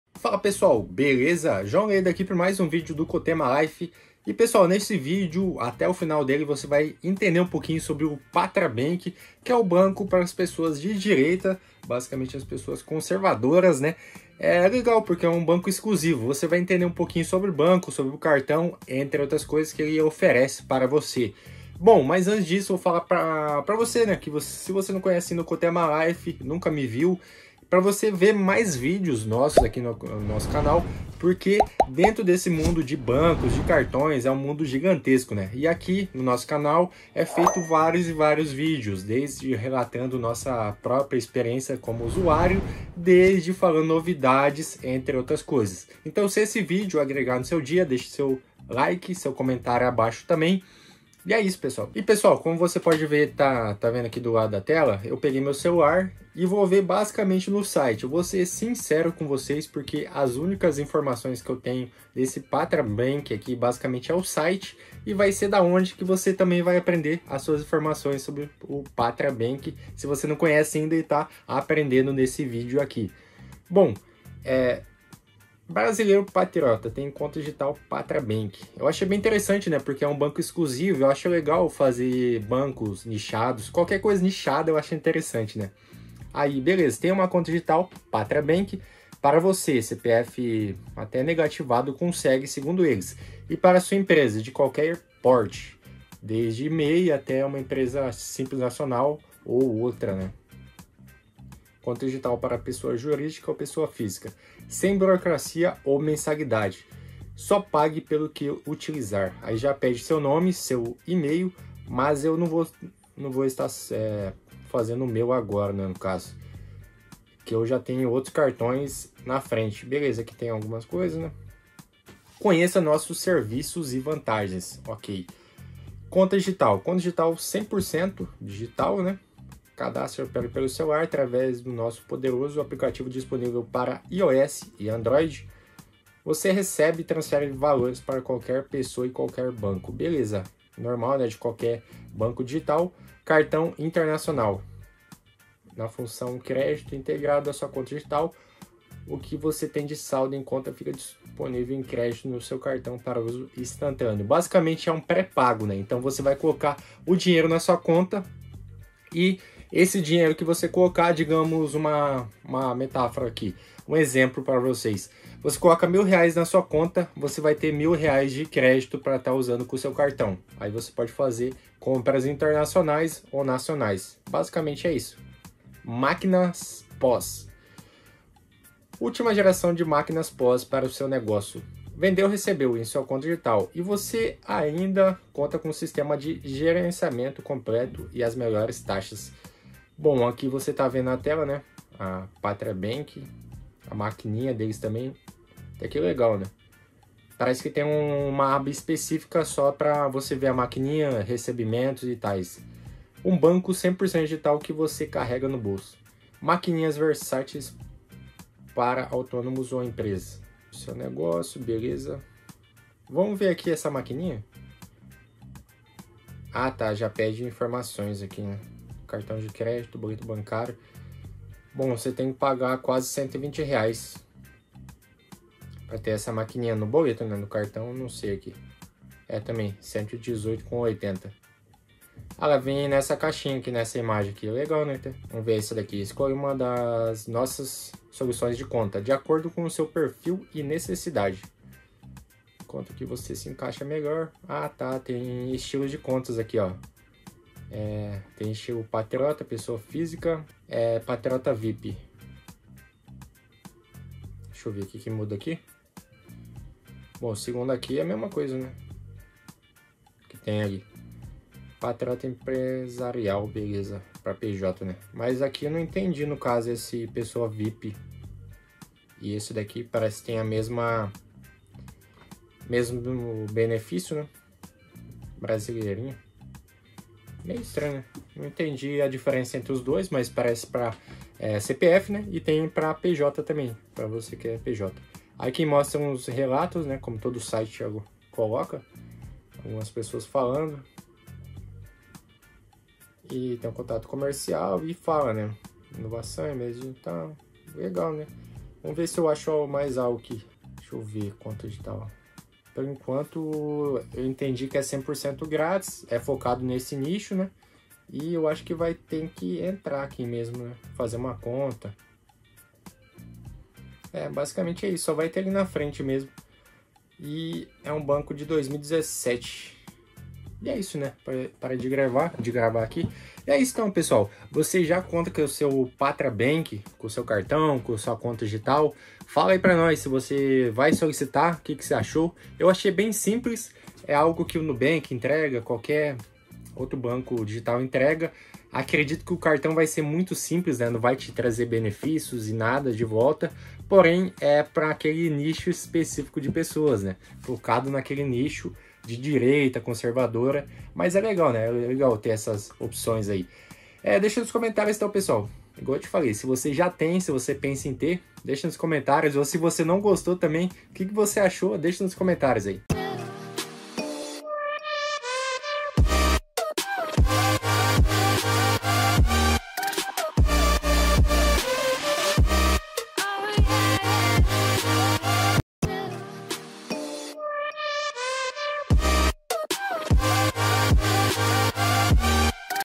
BANCO DIGITAL DOS PATRIOTAS | TUDO SOBRE O PÁTRIA BANK
Se você é liberal na economia e conservador nos costumes, precisa conhecer o pátria bank o bando digital dos conservadores (a direita)
No vídeo eu falo todas informações que temos até o momento desse banco digital!

🎁 Ganhe R$20
(ou use o código: joalei910)

✳️✳️ PEÇA SEUS CARTÕES PELOS LINKS ABAIXO E AUMENTE  A SUA CHANCE DE APROVAÇÃO ✳️✳️